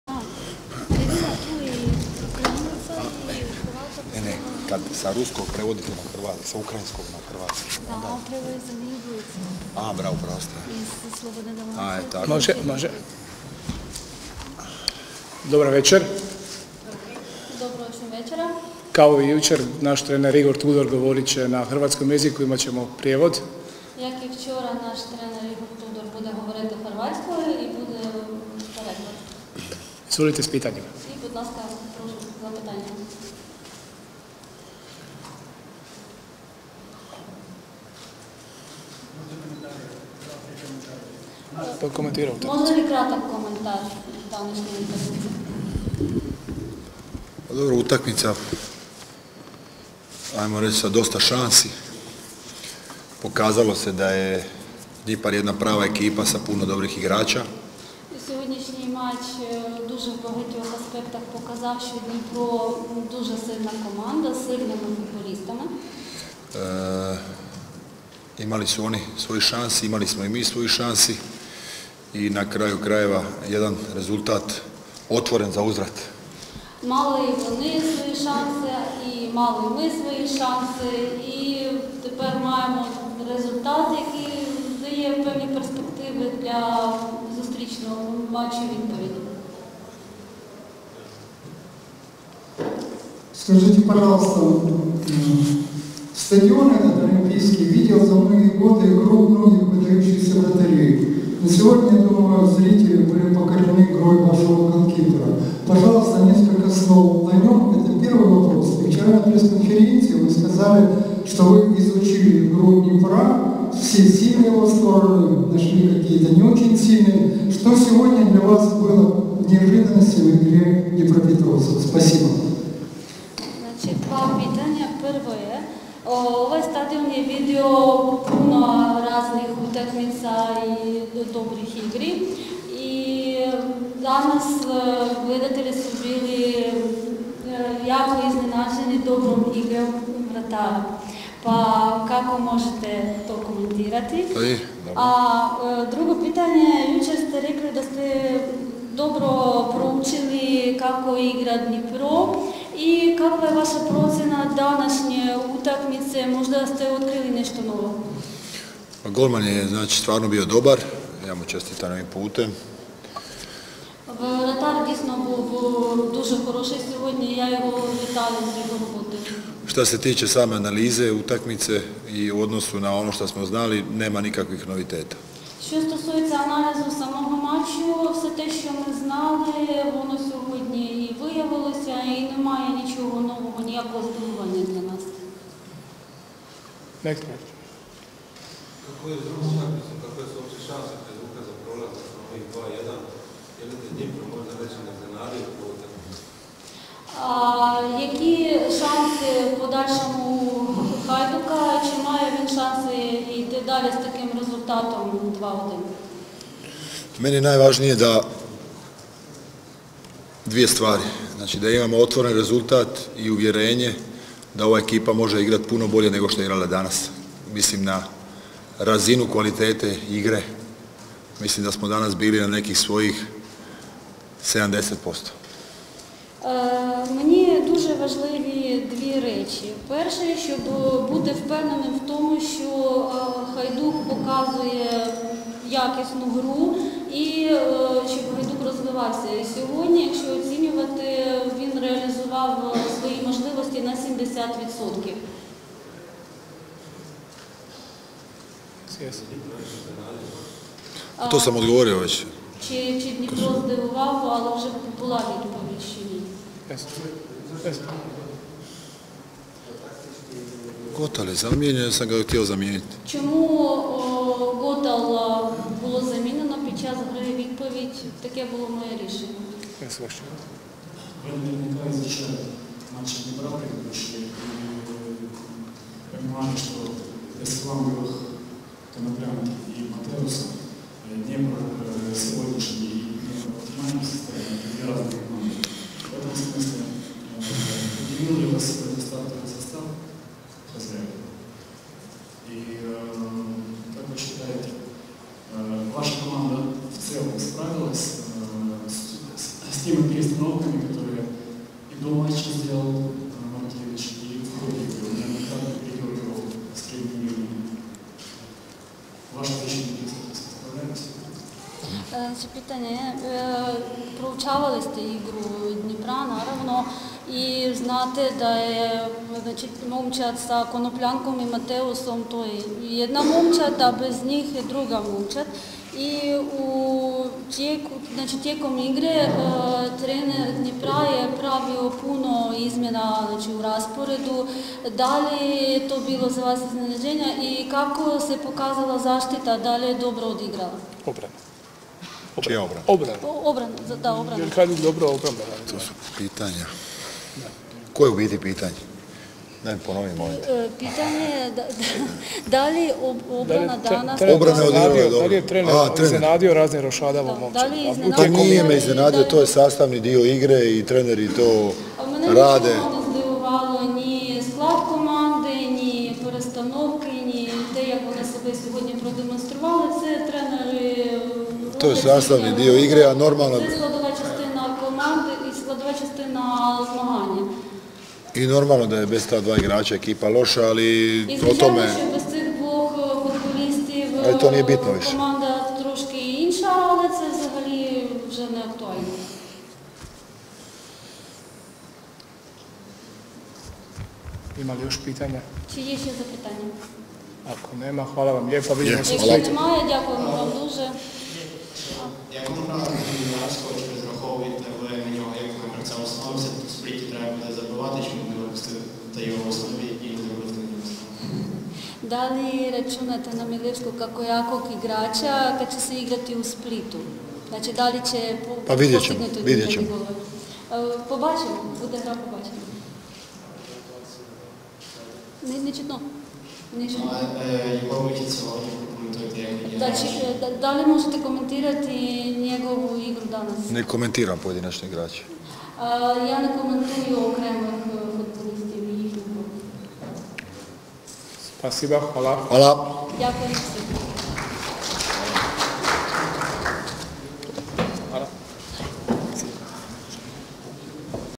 Послематчевая пресс-конференция Игора Тудора (Днепр-Хайдук)
Игор ТУДОР: «Днепр ничем не удивил»
«Днепр» - «Хайдук». Послематчевая пресс-конференция